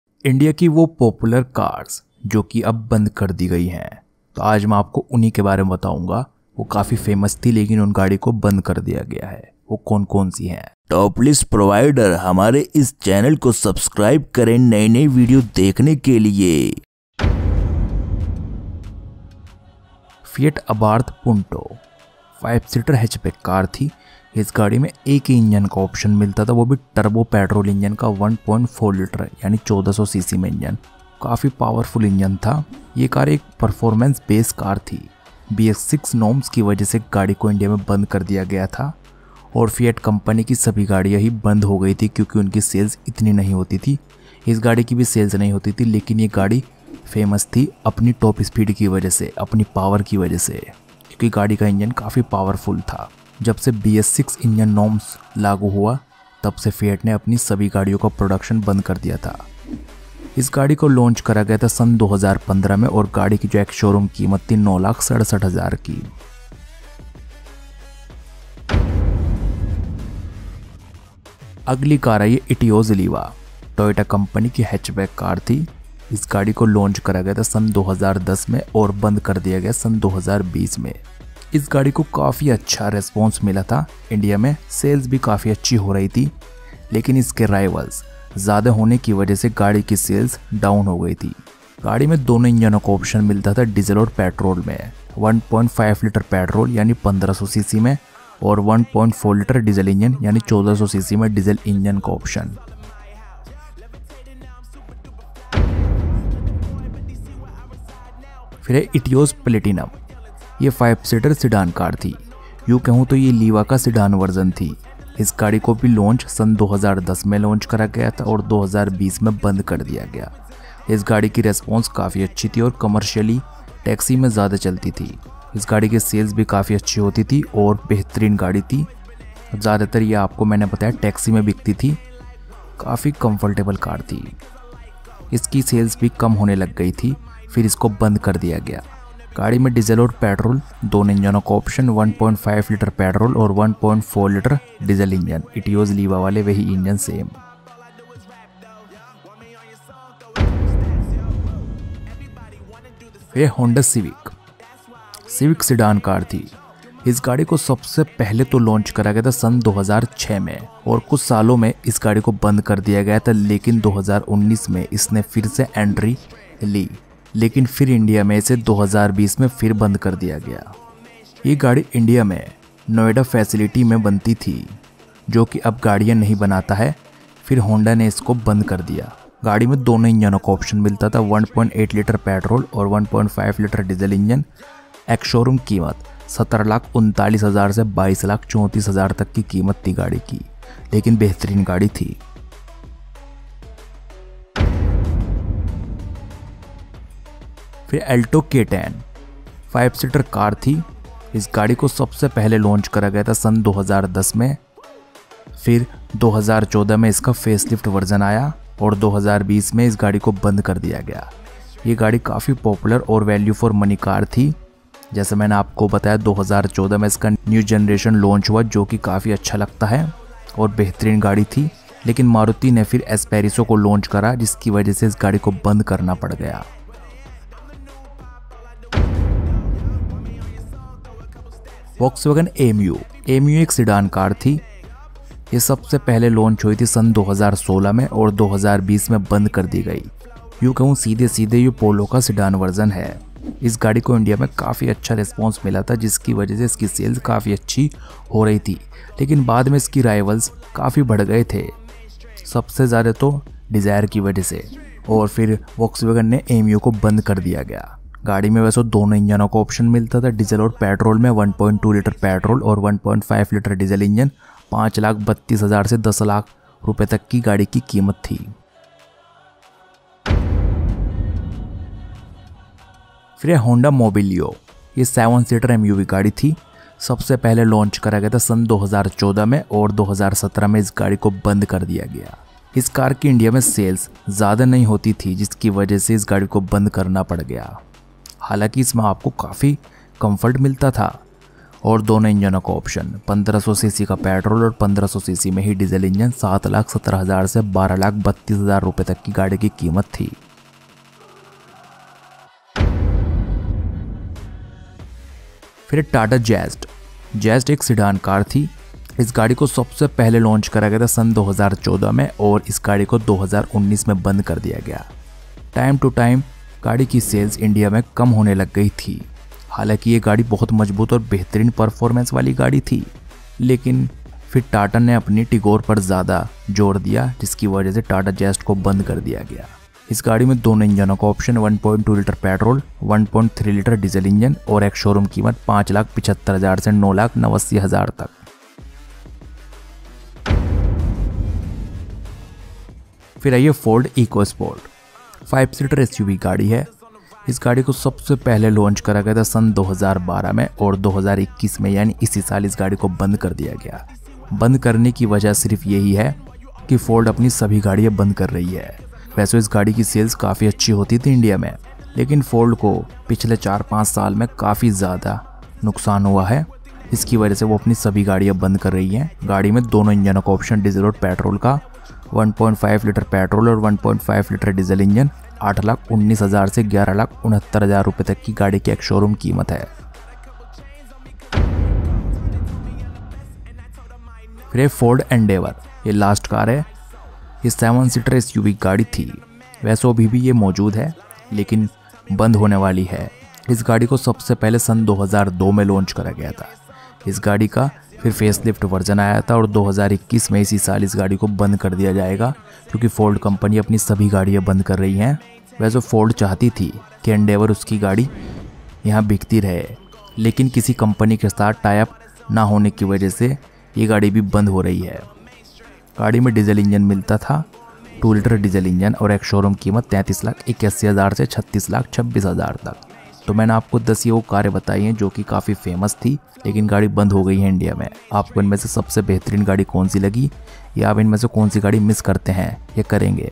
बंद हो चुकी ये प्रसिद्ध गाड़ी | Top 10 Popular Discontinued Cars 2021
बंद हो चुकी ये प्रसिद्ध गाड़ी
Top 10 Popular Discontinued Cars 2021
बंद हुई ये गाड़िया

Popular Cars That are Discontinued

Abarth Punto   =- Alto K10
Etios Liva     =- Ameo
Etios Platinum =- Mobilio
Civic          =- Zest
EcoSport       =- Endeavour